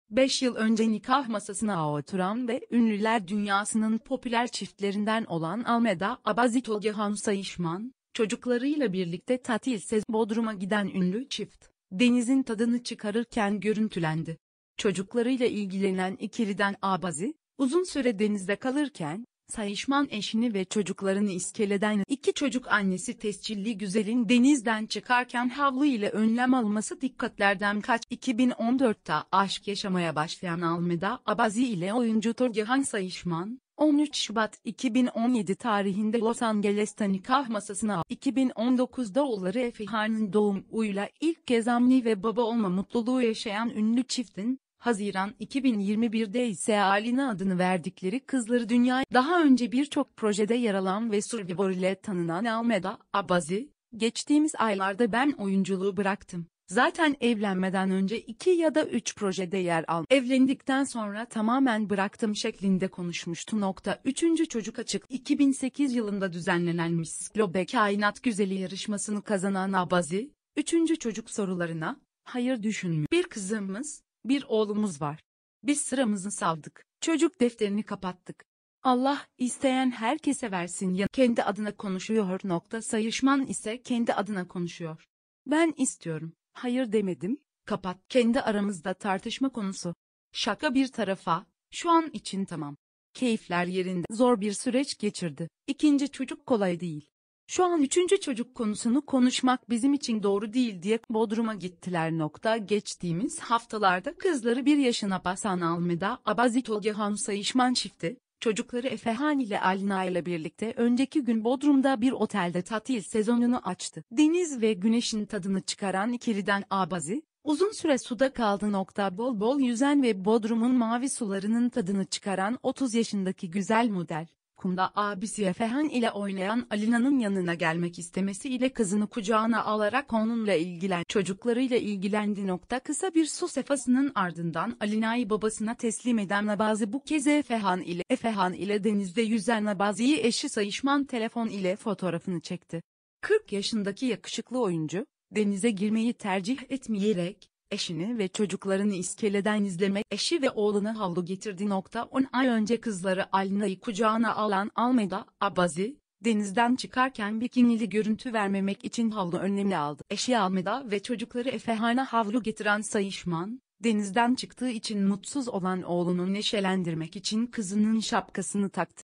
📷Sayışman Ailesi tatilde! Almeda'dan havlulu önlem
Beş yıl önce nikah masasına oturan ve ünlüler dünyasının popüler çiftlerinden olan Almeda Abazi-Tolgahan Sayışman, çocuklarıyla birlikte tatil sezonunu açtı. Bodrum'a giden ünlü çift, denizin tadını çıkarırken görüntülendi. Çocuklarıyla ilgilenen ikiliden Abazi, uzun süre denizde kalırken; Sayışman eşini ve çocuklarını iskeleden izledi. İki çocuk annesi tescilli güzelin denizden çıkarken havlu ile önlem alması dikkatlerden kaçmadı.